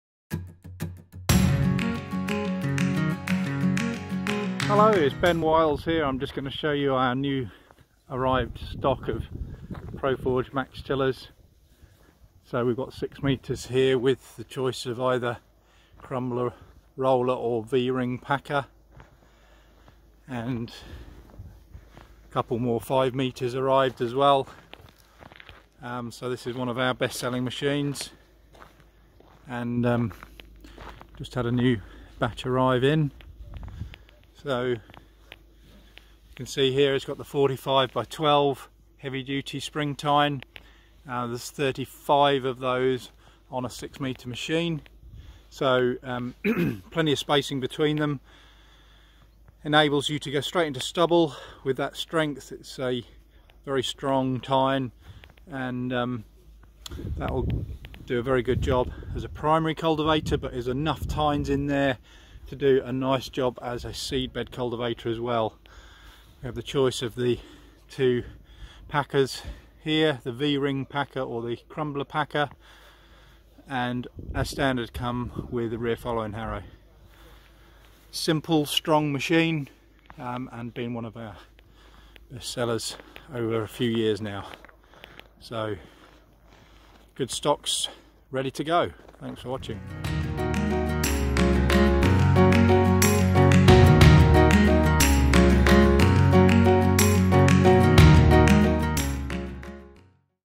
Maxtilla Cultivators In Stock at the Bourne Site!
We've just had delivery of a fresh batch of Proforge Maxtilla cultivators - a fantastic, best-selling cultivator from the Proforge range.

With stock available in 5 or 6 metre widths, this is a brilliant choice for either primary or secondary cultivation this season.